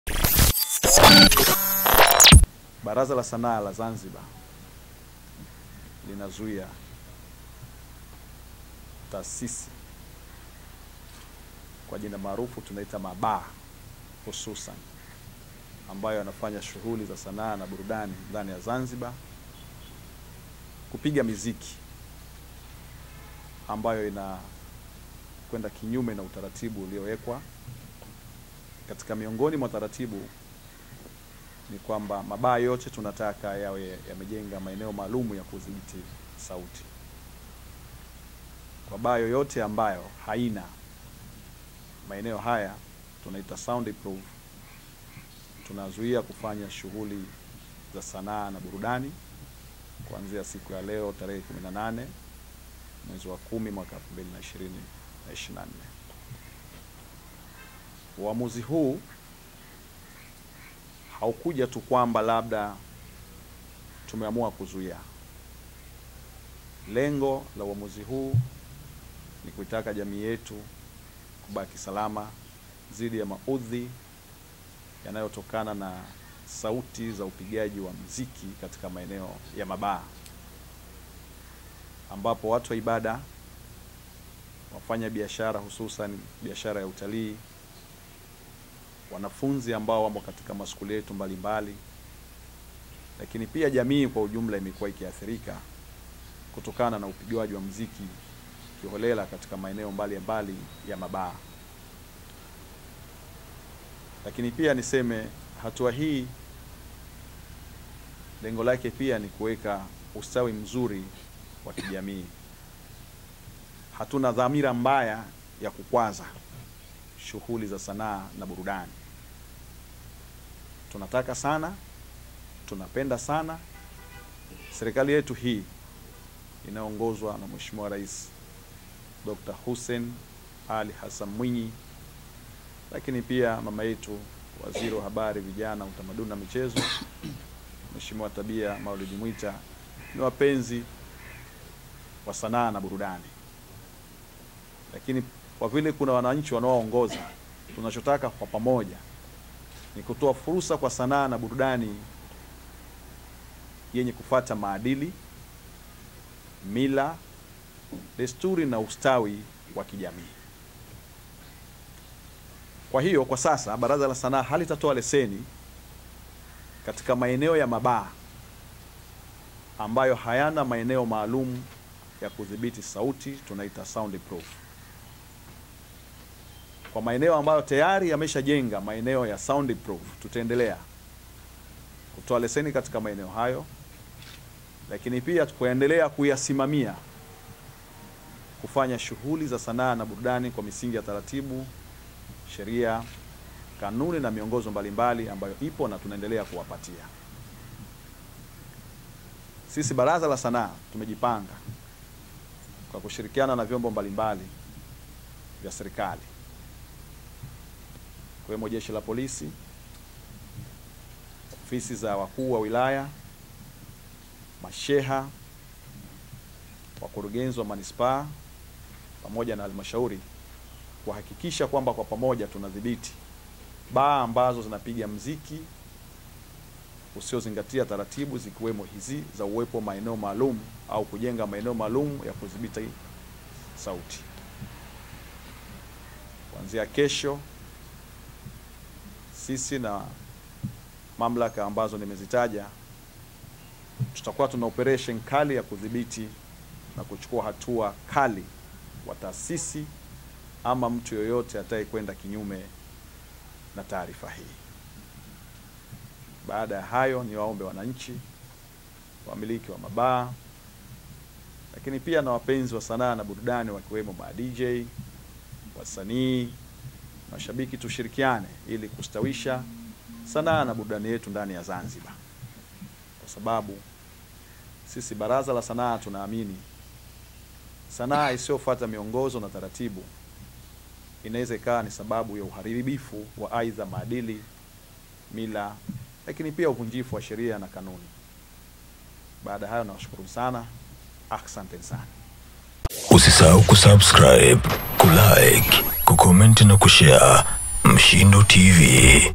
ZANZIBAR MUZIKI WAZUILIWA, KATIBU MTENDAJI WA BASSFU ASEMA MUZIKI BILA SOUND PROOF MABAA NI KERO
Kwa Habari zaidi za Burudani /Michezo / Siasa / KiJamii na Utamaduni Usisite Kutembelea MSHINDO tv ONLINE -
Wasiliana Nasi Kupitia

Bunge bungeni Tanzania Uchumi wa buluu zanzibar